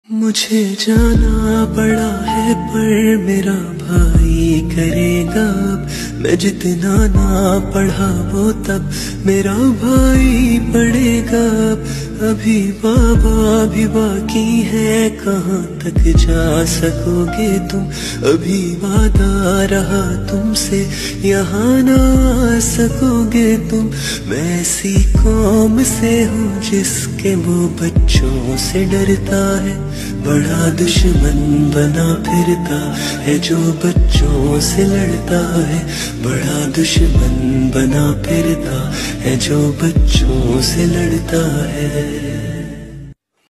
BLACK DAY 🖤🥺 | 16th DECEMBER
Today is black day
Peshawar school attack
Our hero's always in our hearts